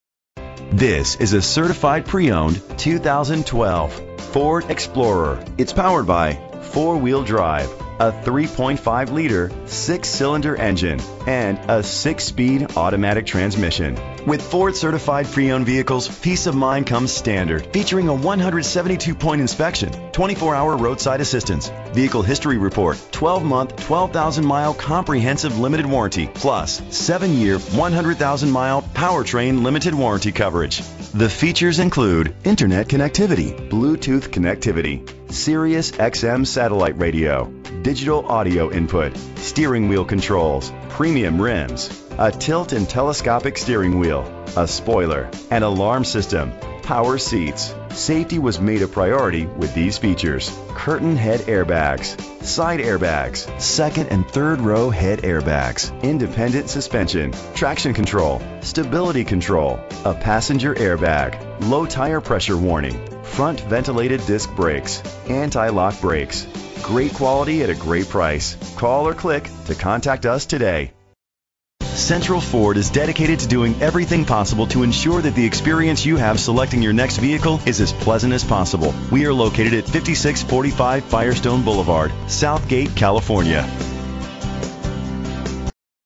2012 Ford Explorer - SOUTH GATE CA
Phone:
Year: 2012
Make: Ford
Model: Explorer
Trim: XLT
Engine: 3.5 liter 6 cylinder 24 valve
Transmission: 6-Speed Automatic
Color: Sterling Gray Metallic
Mileage: 58073
Address:
5645 FIRESTONE BLVD